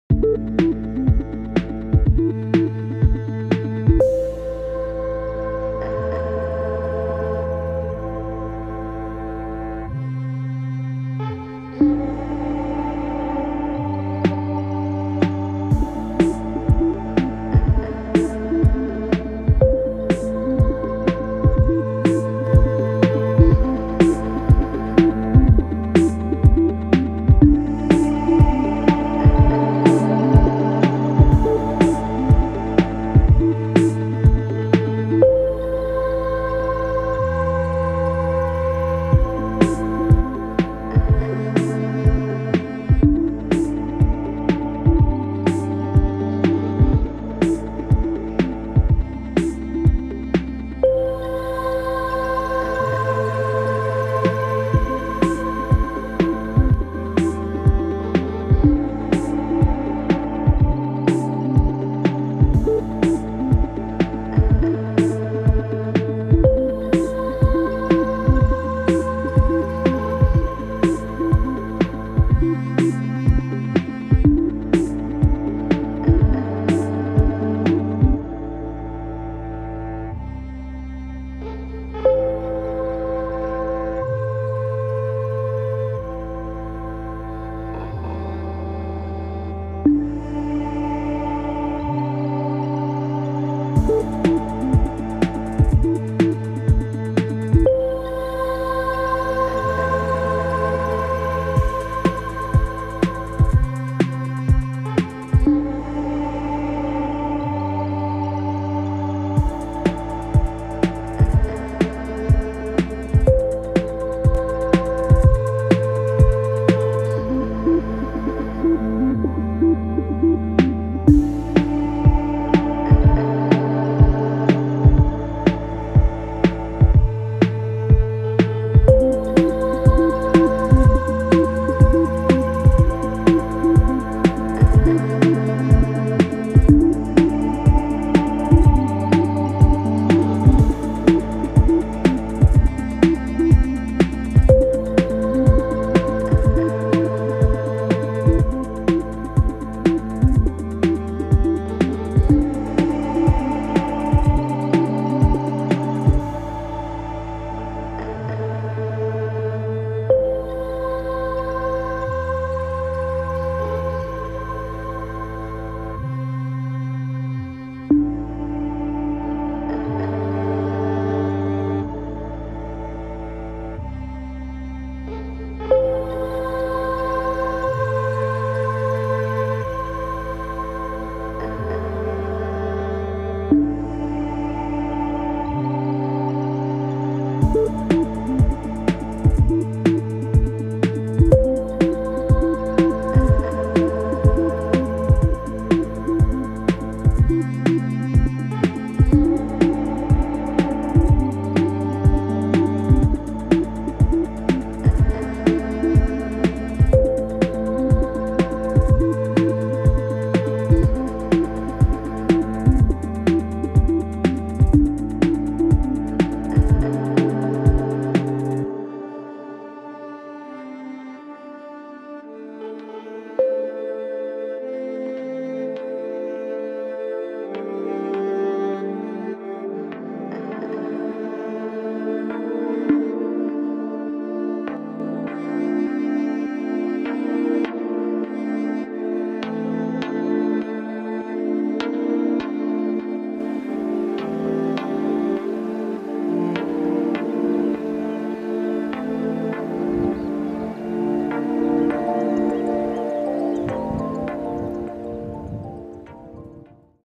Relaxing Sleep Music and Night Nature Sounds: Soft Crickets, Beautiful Piano, Deep Sleep Musac
Relaxing sleep music for deep sleeping and stress relief. Fall asleep to beautiful nature videos and use the relaxing music relaxing Zen Music with Water Sounds • Relaxing Sleep Music and Night Nature Sounds: Soft Crickets, Beautiful Piano, Deep Sleep Music
Dream Sound